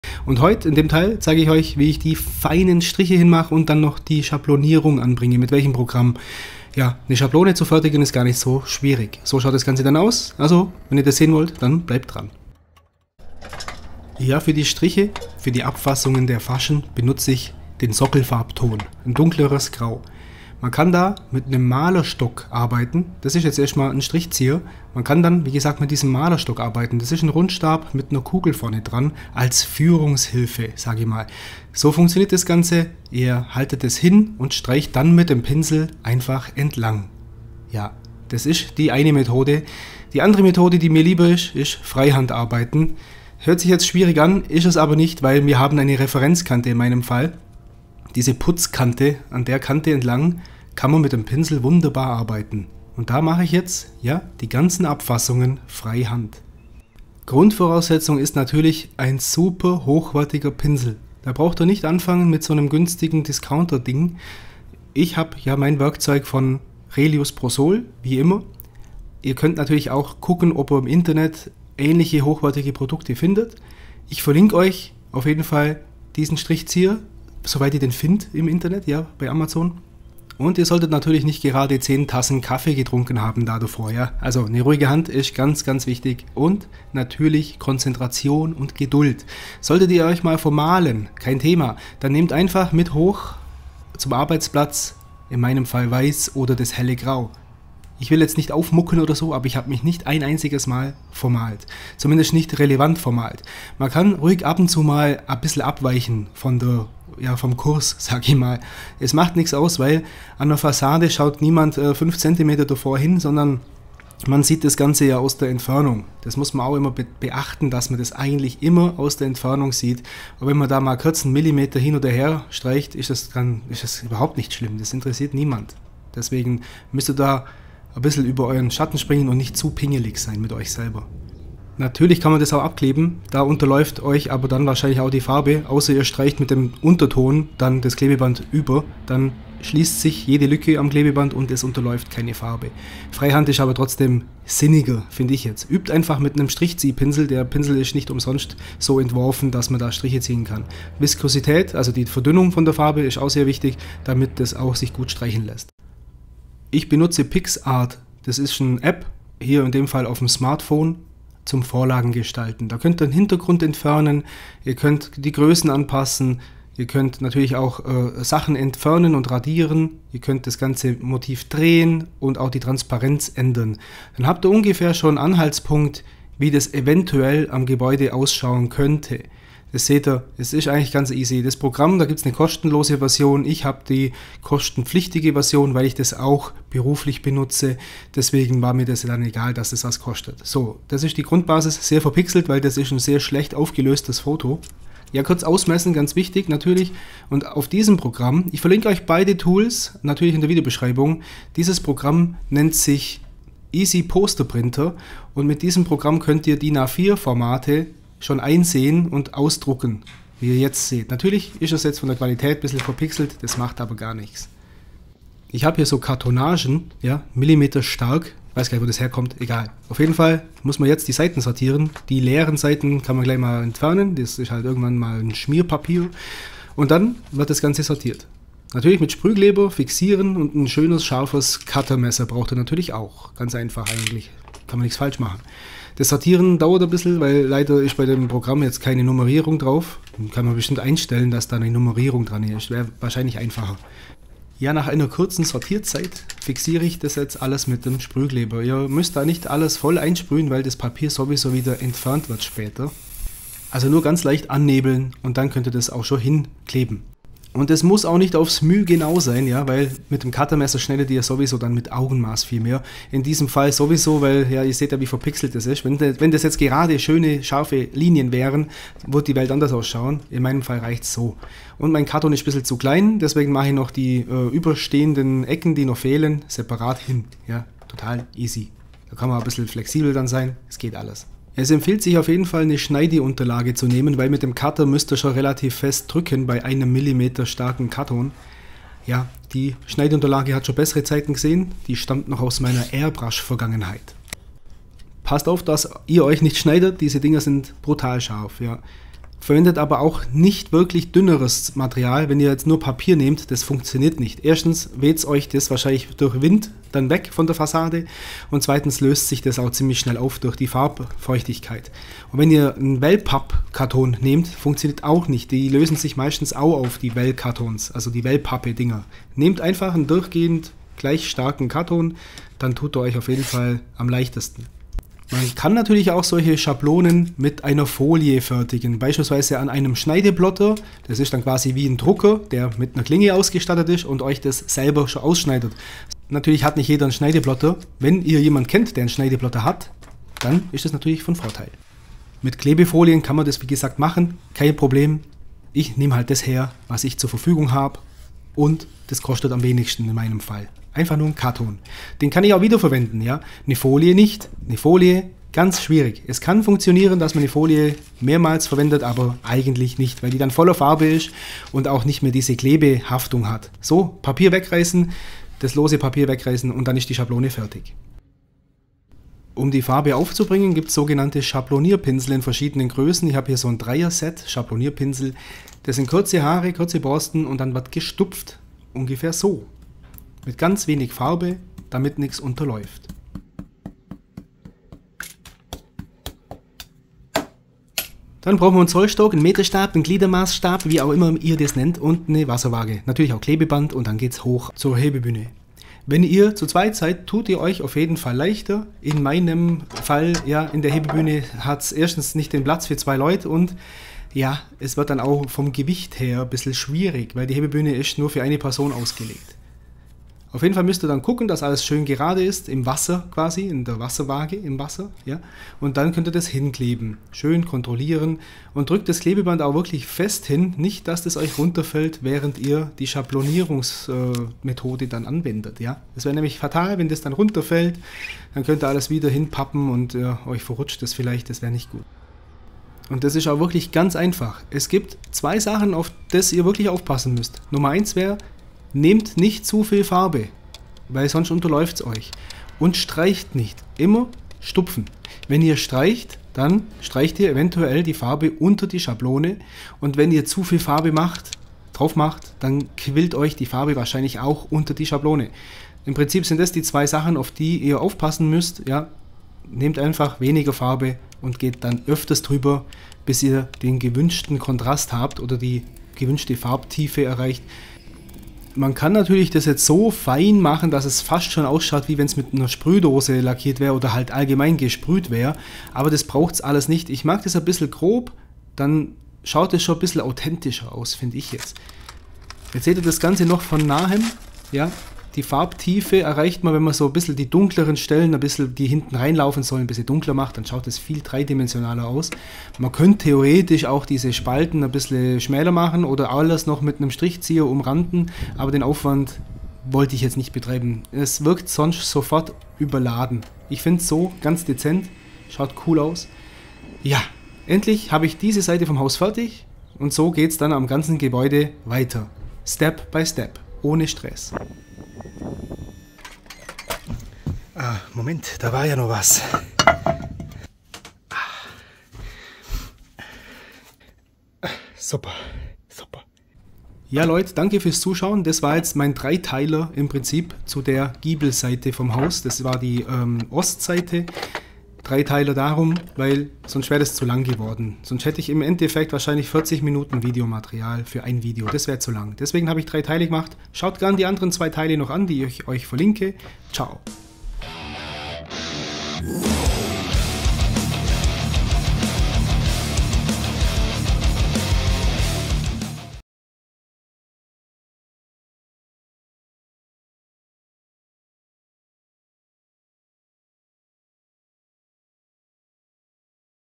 Fassadenmalerei, Wandschablone erstellen, Abfassungen linieren, Teil 3
Dies ist der dritte Teil meiner Fassaden Gestaltungs-Reihe.
Heute mache ich den letzten Feinschliff.
Striche ziehen und einen keltischen Knoten auf schablonieren.

⚠️ACHTUNG⚠️gereinigt wurde ohne Chlor. Bei einer Fassadenreinigung muss überschüssiges Chlor, wenn dieses verwendet wird, aufgefangen werden. Verwendet also biologisch-abbaubare Produkte. Mein Regenfallrohr geht übrigens zu einer Zisterne, die mit einem Feinstaub Filter ausgestattet ist, dort werden Holzstaub und andere Partikel vom Wasser getrennt.
Übrigens wird auch das Abwasser welches beim Werkzeug reinigen entsteht, bei einem befreundeten Malermeister entsorgt. Quasi im Strobber Waschsystem

*Bonum Schrägstrichzieher
*STORCH Schräg-Strichzieher
*Maler Stab
*1mm Pappe
*Goldband Abklebeband
*STAHLWERK Zollstock Gliedermaßstab Meterstab
*STAHLWERK Wasserwaage
*Schneidematte 90 x 60 cm

damit ihr Projekt Storys sehen könnt.

Musik von Nasvai.